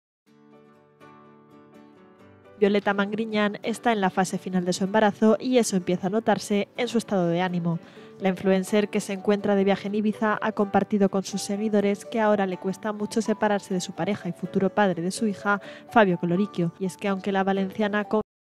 Violeta Mangriñán cuenta cómo está viviendo la etapa final de su embarazo
Violeta Mangriñán está en la fase final de su embarazo y esto empieza a notarse en su estado de ánimo. La influencer, que se encuentra de viaje en Ibiza, ha compartido con sus seguidores que ahora le cuesta mucho separarse de su pareja y futuro padre de su hija, Fabio Colloricchio.